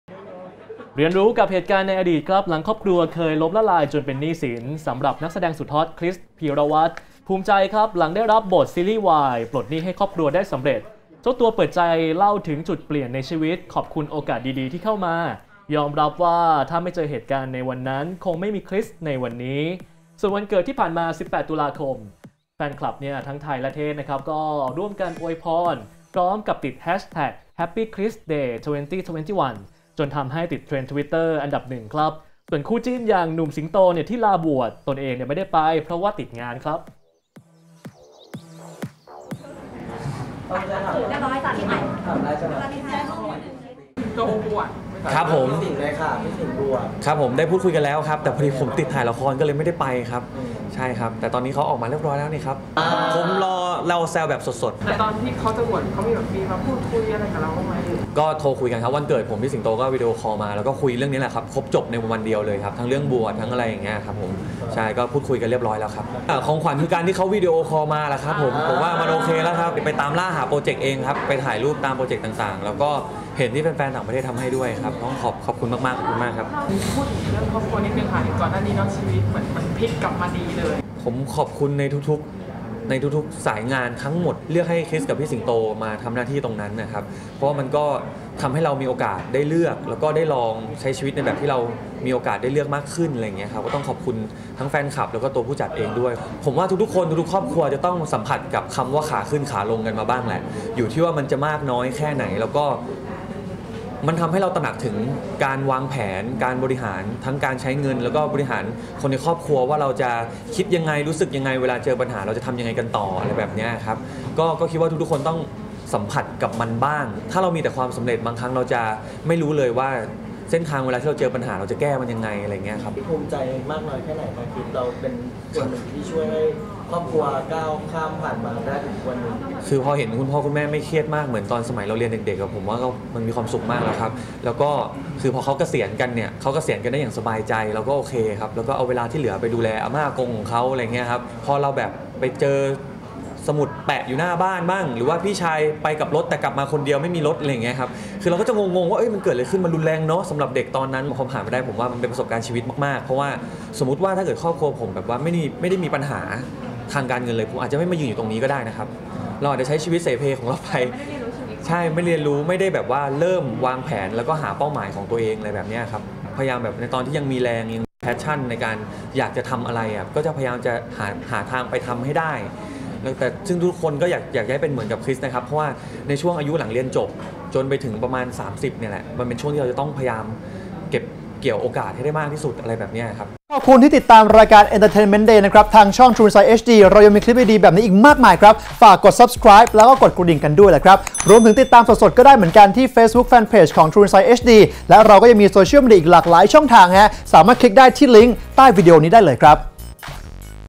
“คริส พีรวัส” เผยซีรีส์วายพลิกชีวิตปลดหนี้ให้ครอบครัว
Entertainment Day_051164

เรียนรู้กับเหตุการณ์ในอดีตหลัง ครอบครัวเคยล้มละลายจนเป็นหนี้สิน สำหรับนักแสดงสุดฮอต คริส พีรวัส ภูมิใจหลังได้รับบทซีรีส์วายปลดหนี้ให้ครอบครัวได้สำเร็จ
เจ้าตัวได้เปิดใจเล่าถึงจุดเปลี่ยนในชีวิตขอบคุณโอกาสดีๆที่เข้ามา ยอมรับว่าถ้าไม่เจอเหตุการณ์ในวันนั้นคงไม่มี คริส ในวันนี้ ส่วนวันเกิดที่ผ่านมา 18 ตุลาคม แฟนคลับทั้งไทยและเทศร่วมอวยพร พร้อมแฮชแท็ก Happykristday2021จนทำให้ติดเทรนด์ทวิตเตอร์อันดับหนึ่ง ส่วนคู่จิ้นอย่างหนุ่ม สิงโต ที่ลาบวช ตนเองไม่ได้ไป เพราะติดงาน